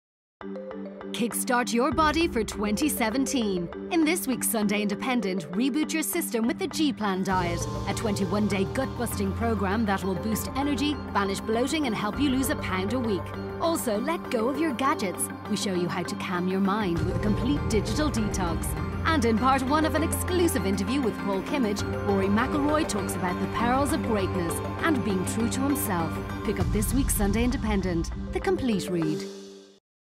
The G-Diet - Lose a pound a week
Kickstart your body for 2017.
In this week’s Sunday Independent:, reboot your system with the G Plan diet - a 21-day gut-busting programme that will boost energy, banish bloating and shift those Christmas pounds.
Plus – ditch the gadgets and declutter your mind with our expert tips to a Digital Detox And in part one of an exclusive interview with Paul Kimmage, Rory McIlroy talks about the perils of greatness and being true to himself.
Pick up this week’s Sunday Independent.
The Complete Read